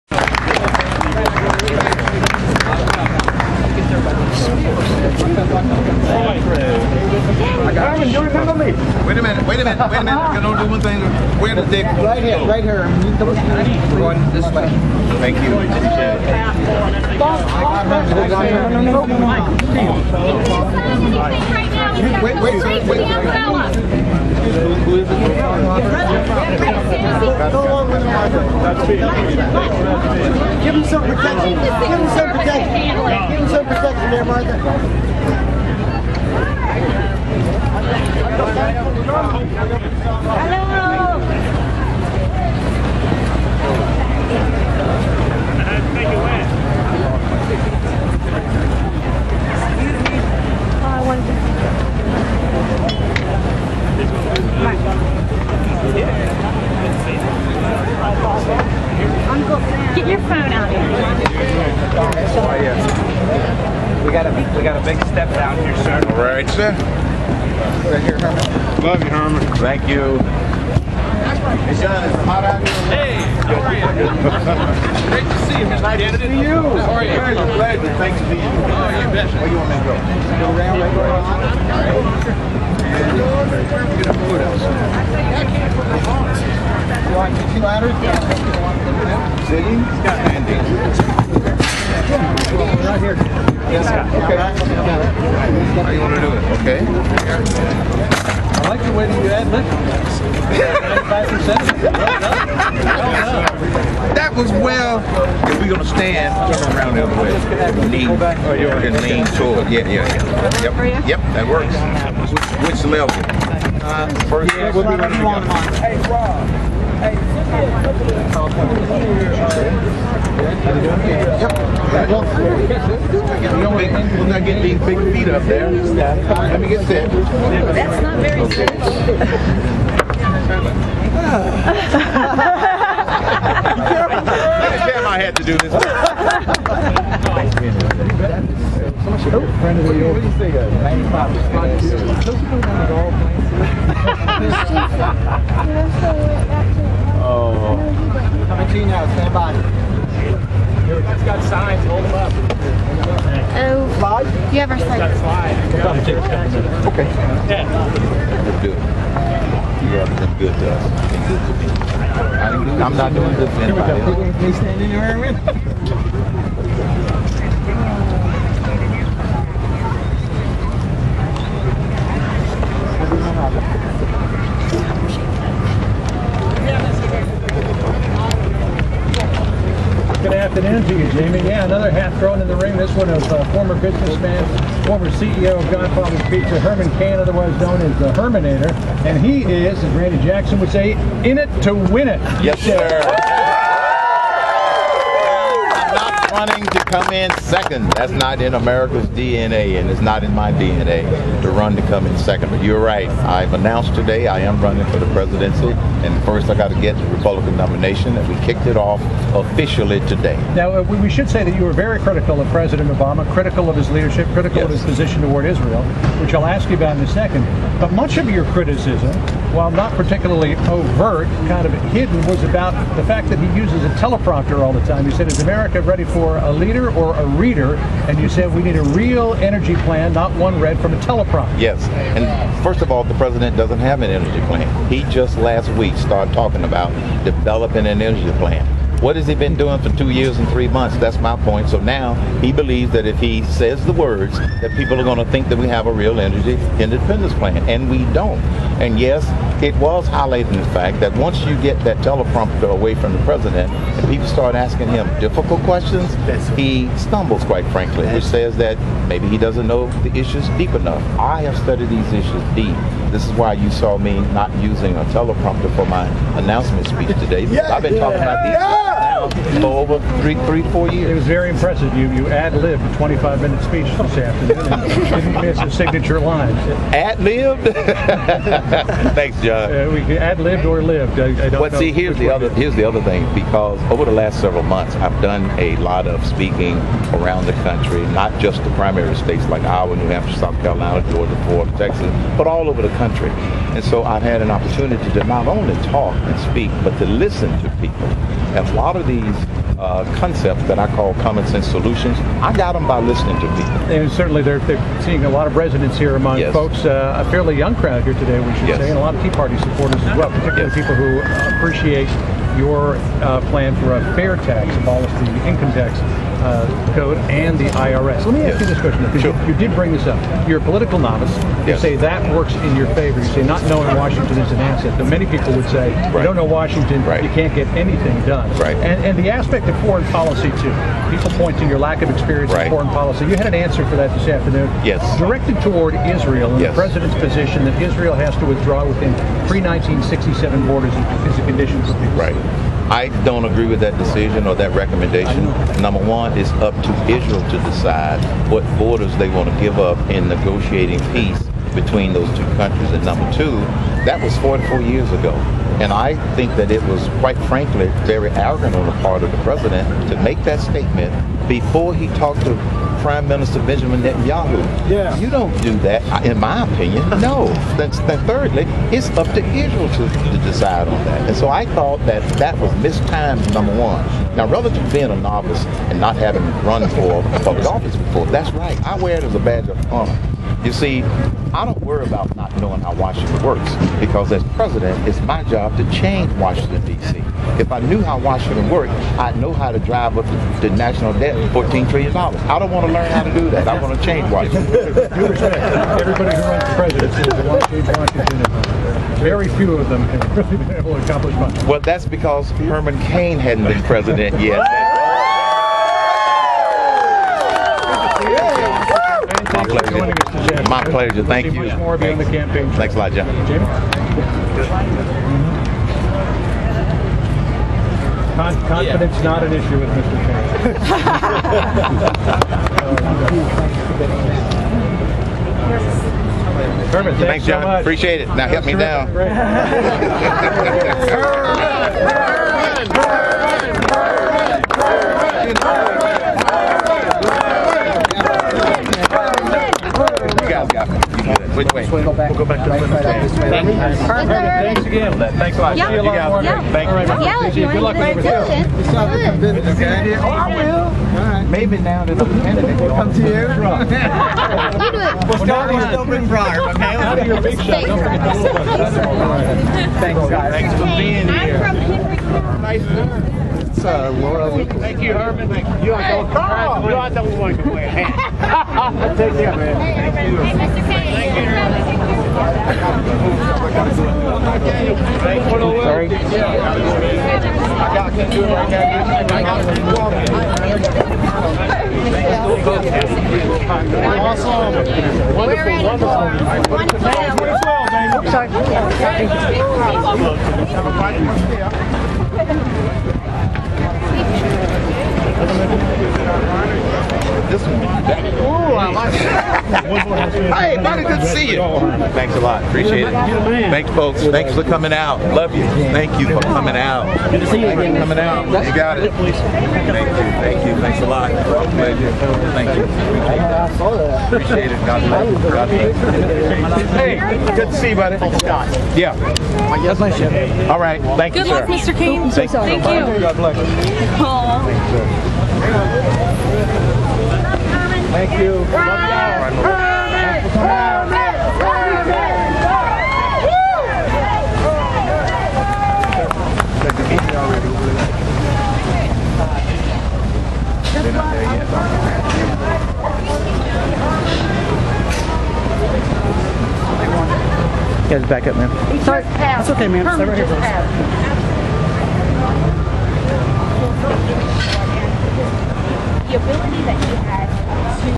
Herman Cain Interviewed By Fox News' John Roberts
Recorded after Herman Cain's Announcement Rally at Atlanta's Olympic Centennial Park on May 21, 2011.  I recorded this video from the other side of where the Fox News camera was recording.  I also recorded for quite some time before and after they started recording, capturing some candid banter between the crowd and Mr. Cain as well as between Mr. Roberts and Mr. Cain.  For comparison, here is the link to the official Fox News video  Mr. Roberts mentions Herman Cain's speaking ability and refers to his 25 minute speech.